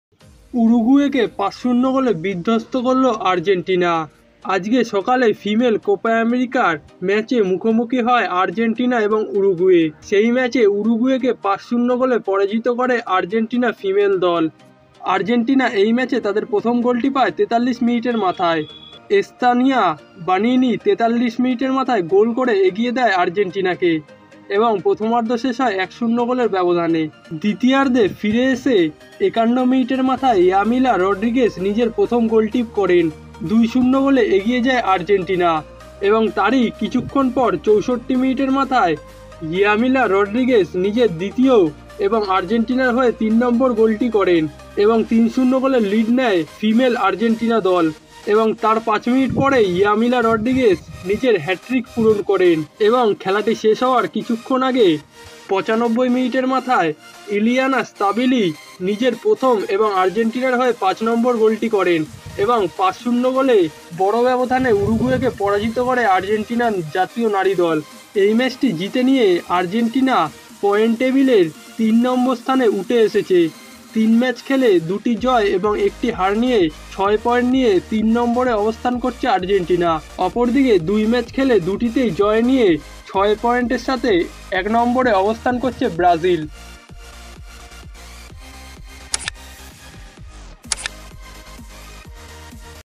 আর্জেন্টিনার বড়ো জয়💥❗উরুগুয়েকে ৫-০ গোলে হারালো আর্জেন্টিনা💥১৮ মিনিটে হ্যাটট্রিক করলেন ইয়ামিলা💥
Hero isl semi final matches date and time table🔥🔥/Jamshedpur FC vs Kerala blasters semi final match

ধন্যবাদ❤️❤️